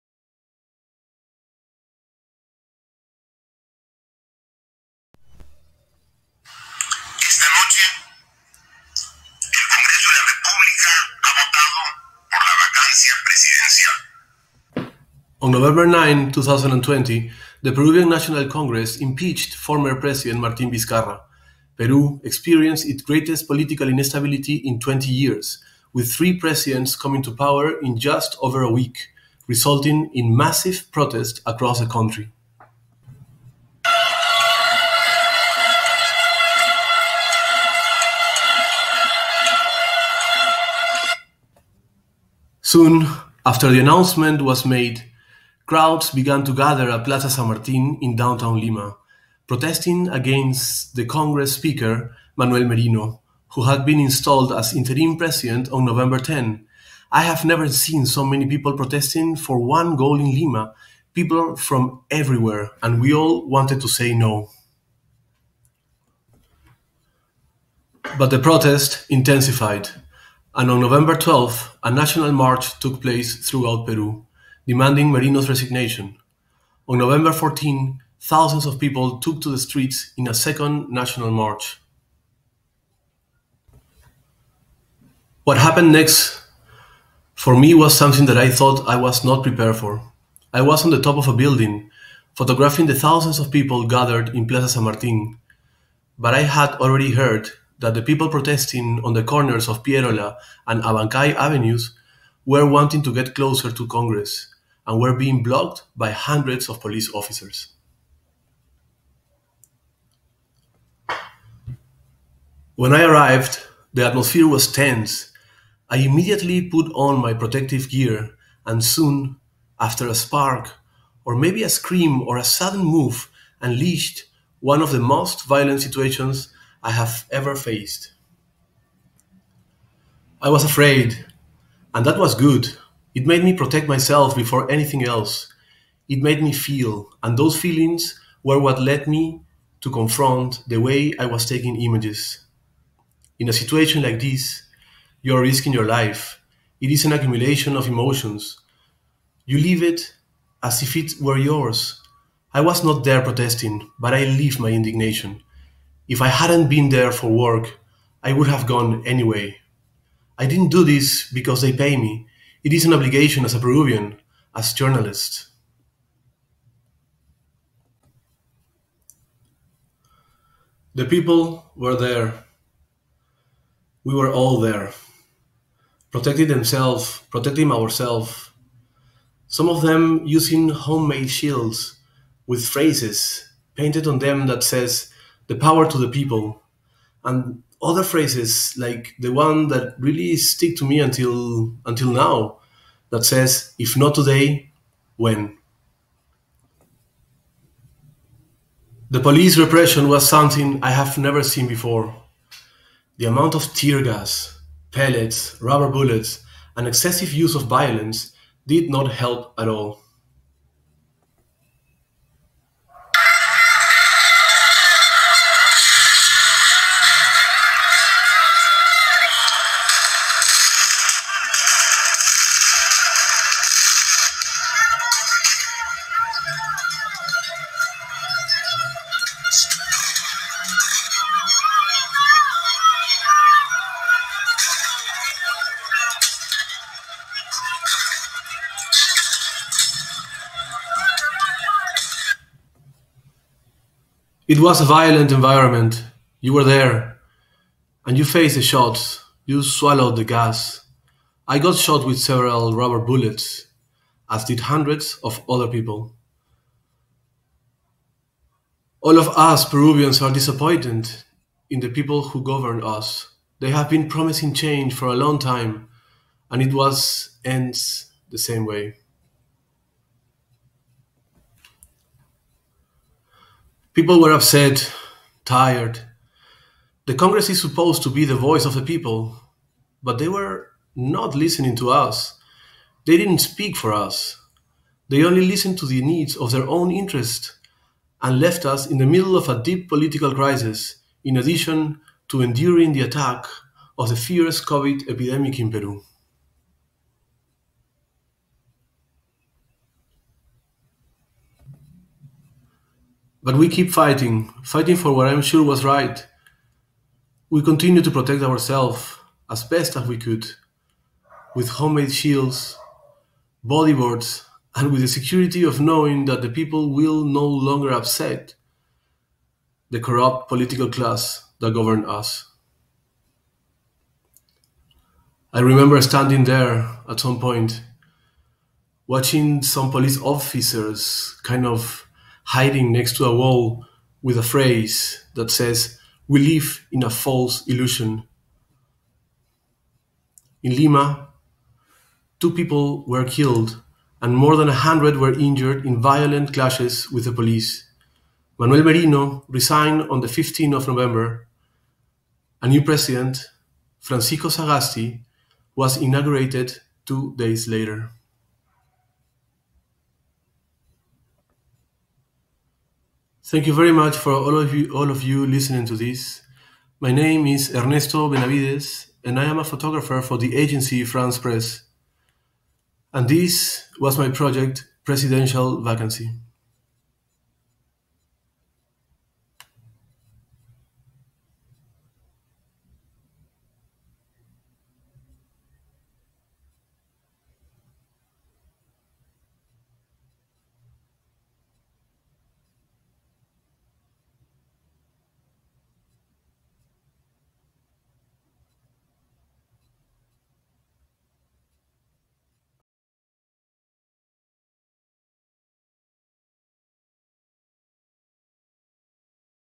World Press Photo Festival 2021 | Winners Speak: Ernesto Benavides
Ernesto Benavides shares the story behind 'Presidential Vacancy,' awarded 2nd prize in the Spot News, stories category of the 2021 World Press Photo Contest.

The video was part of the 'Show and Tell: Winners Speak' session during the online World Press Photo Festival 2021.

With PechaKucha, the winners will show twenty images for twenty seconds, and dive deeper into their winning stories. In 6 minutes and 40 seconds, you will learn about the process they went through, the hurdles they overcame and the most impactful moments they experienced along the way.